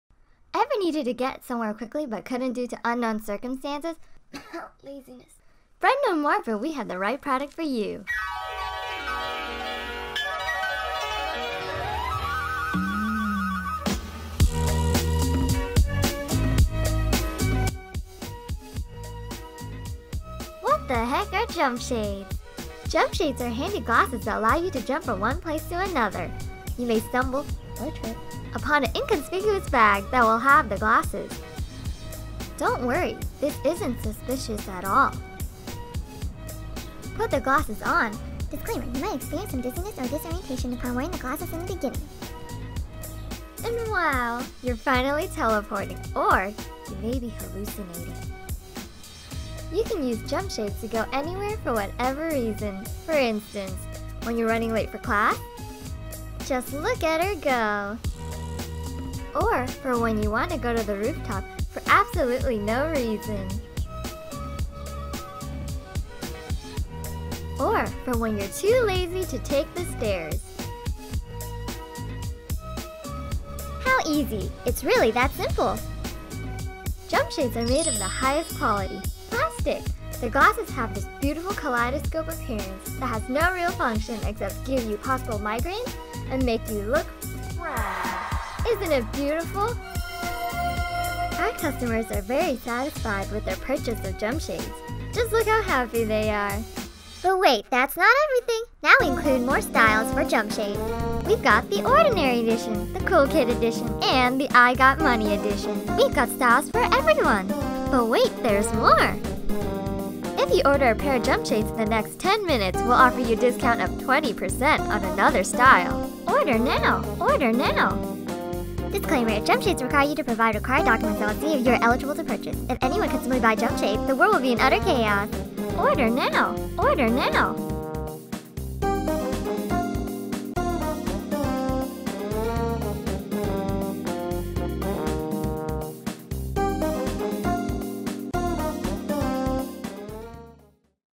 Jumpshades
CTVA 464 Final project (2018)
A new way of travel for the common folk!